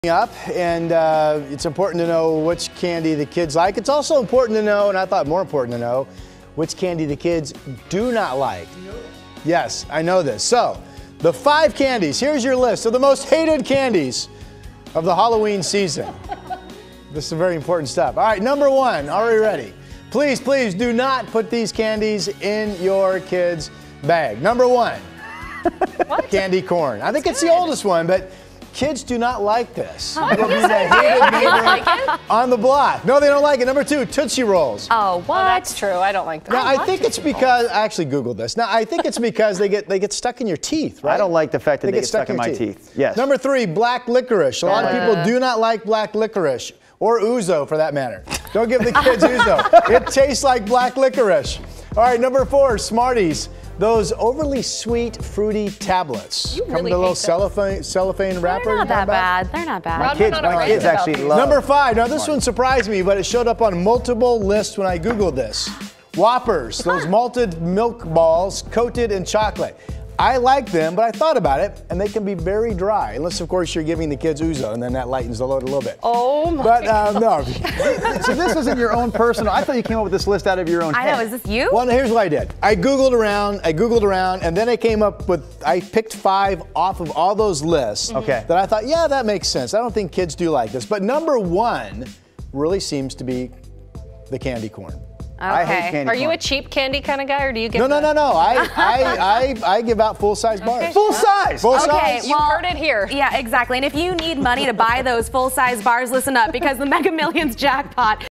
Rod Hill lists the most unpopular trick-or-treat candies
Here are the Halloween trick-or-treat candies kids hate the most.